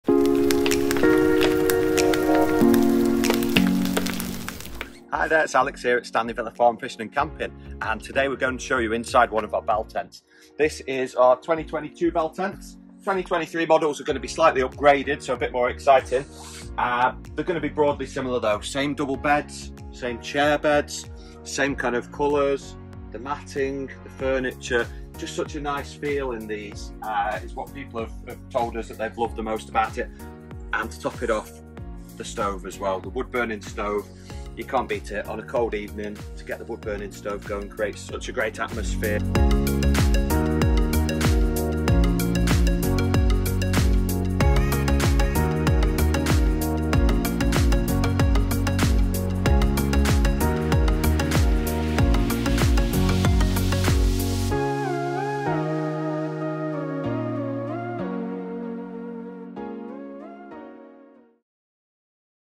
Whats in the Bell Tents - Stanley Villa Farm Fishing & Camping
We have 10 New Bell Tents, all the same in size, set out in their own magical little community!

Each Bell Tent comes with a double bed & mattress and 2 Single Chair Beds. An additional airbed can be hired if required.

Bell Tents sleep 2 adults (max) and up to three children. You just need to bring your own bedding.

Bell Tents come with a wood burning stove inside to keep you toasty warm and help create a magical atmosphere.

Bell Tents are all sat on wooden decking and come with stylish home furnishings such as bedside tables, matting, cushions and of course bunting, and fairy lights!

Our Bell Tents don’t have electricity so it’s an authentic camping experience. If you do need to charge phones or plug in your hairdryer, there is plenty of power available in the nearby Lakeside Lodge. Or you can hire a battery pack on site if you’d rather charge your device in your Bell Tent.

Bell Tents each come with a firepit. Wood can be purchased on site.

Check in from 3pm until 8pm.

Check out 10.30am.

Early Arrival & Late Check out available at an additional cost and can be added during the booking process.